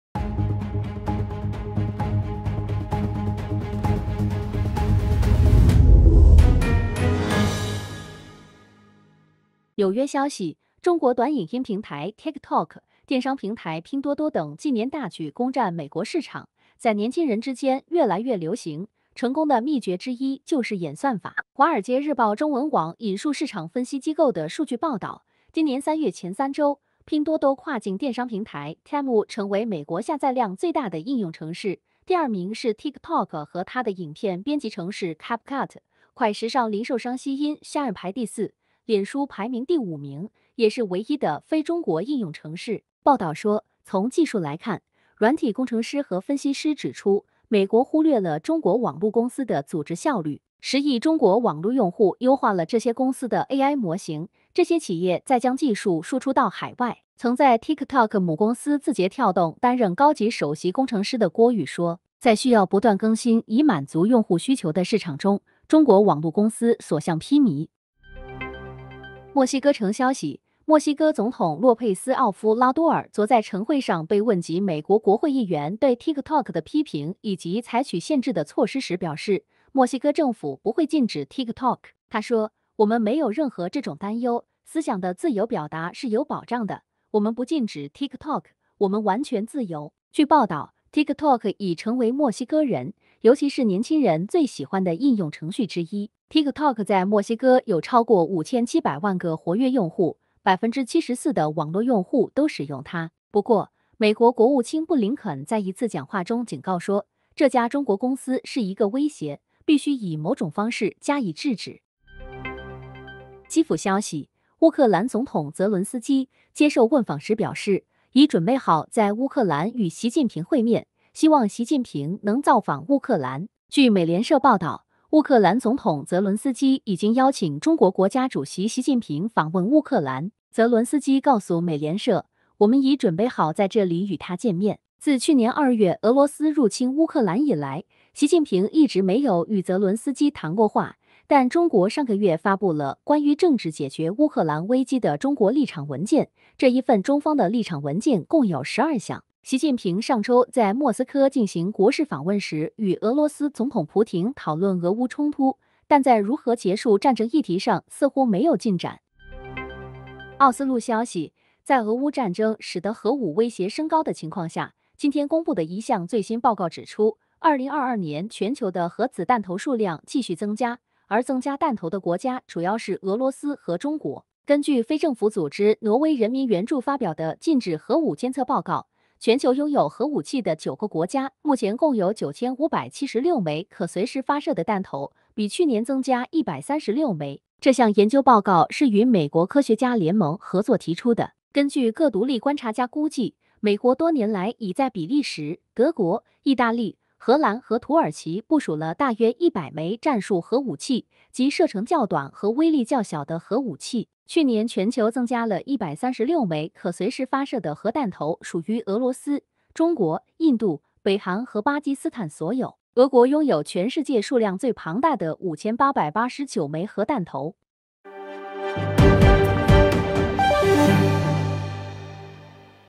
新聞快訊 | 墨西哥總統：我們不禁止TikTok
中國app征服美青年 美媒：演算法是成功秘訣

紐約消息：中國短影音平台Tiktok、電商平台拼多多等近年大舉攻占美國市場，在年輕人之間越來越流行，成功的祕訣之一就是演算法。
華爾街日報中文網引述市場分析機構的數據報導，今年3月前3週，拼多多跨境電商平台Temu成為美國下載量最大的應用程式，第二名是TikTok和它的影片編輯程式CapCut，快時尚零售商希音（Shein）排第四，“臉書”排名第5名、也是唯一的非中國應用程式。報導說，從技術來看，軟體工程師和分析師指出，美國忽略了中國網路公司的組織效率。 10億中國網路用戶優化了這些公司的AI模型，這些企業再將技術輸出到海外。曾在TikTok母公司字節跳動擔任高級首席工程師的郭宇說，在需要不斷更新以滿足用戶需求的市場中，中國網路公司所向披靡。

墨西哥總統：我們不禁止TikTok

墨西哥城消息，墨西哥總統洛佩斯·奧夫拉多爾昨在晨會上被問及美國國會議員對TikTok的批評以及採取限制的措施時表示，墨西哥政府不會禁止TikTok。他說：“我們沒有任何這種擔憂，思想的自由表達是有保障的，我們不禁止TikTok，我們完全自由。”   據報導，TikTok已成為墨西哥人，尤其是年輕人最喜歡的應用程序之一。 TikTok在墨西哥有超過5700萬個活躍用戶，74%的網絡用戶都使用它。不過，美國國務卿布林肯在一次講話中警告說，這家中國公司是一個威脅，必須“以某種方式加以製止”。

澤倫斯基邀習近平訪烏

基輔消息：烏克蘭總統澤倫斯基接受問訪時表示，已準備好在烏克蘭與習近平會面，希望習近平能造訪烏克蘭。據美聯社報導，烏克蘭總統澤倫斯基已經邀請中國國家主席習近平訪問烏克蘭。澤倫斯基告訴美聯社：「我們已準備好在這裡與他見面。」自去年2月俄羅斯入侵烏克蘭以來，習近平一直沒有與澤倫斯基談過話，但中國上個月發布了「關於政治解決烏克蘭危機的中國立場」文件，這一份中方的立場文件共有12項。習近平上週在莫斯科進行國是訪問時 與俄羅斯總統蒲亭討論俄烏衝突，但在如何結束戰爭議題上似乎沒有進展。

2022全球可用核彈頭增 俄中增加最多

奧斯陸消息：在俄烏戰爭使得核武威脅升高的情況下，今天公佈的一項最新報告指出，2022年全球的核子彈頭數量繼續增加，而增加彈頭的國家主要是俄羅斯和中國。根據非政府組織「挪威人民援助」發表的「禁止核武監測」報告，全球擁有核武器的9個國家，目前共有9576枚可隨時發射的彈頭，比去年增加136枚。這項研究報告是與美國科學家聯盟合作提出的。根據各獨立觀察家估計，美國多年來已在比利時、德國、義大利、荷蘭和土耳其部署了大約100枚「戰術」核武器，即射程較短和威力較小的核武器。去年全球增加了136枚可隨時發射的核彈頭屬於俄羅斯、中國、印度、北韓和巴基斯坦所有；俄國擁有全世界數量最龐大的5889枚核彈頭。